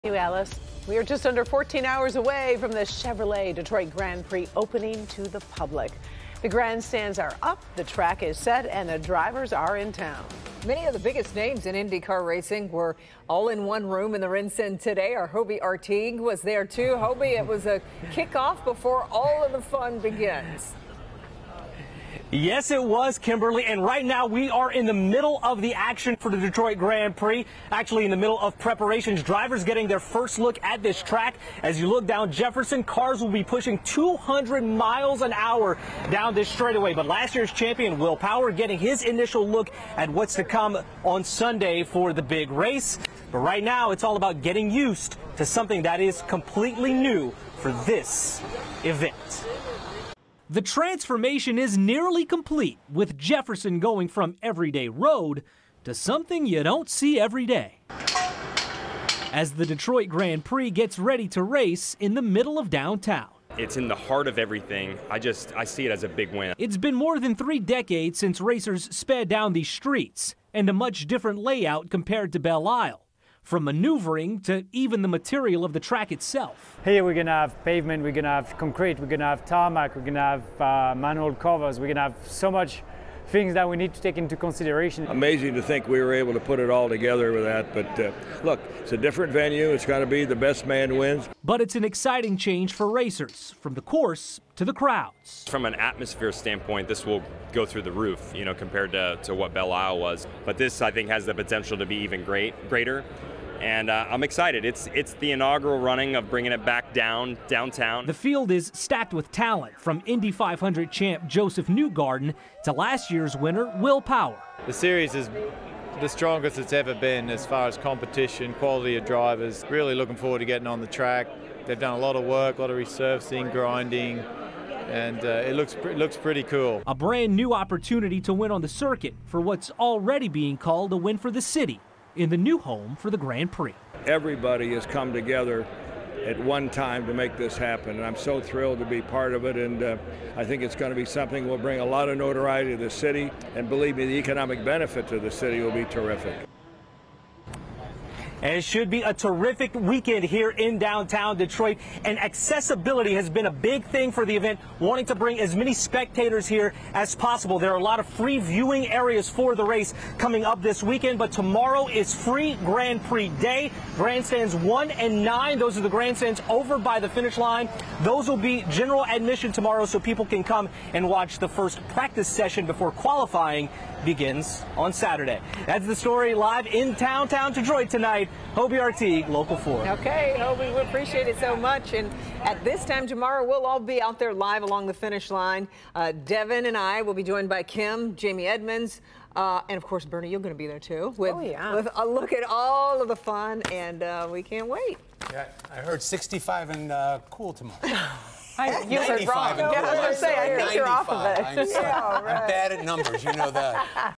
The kickoff to the Detroit Grand Prix in Downtown Detroit
The grandstands are up and the racers are in town for the Detroit Grand Prix.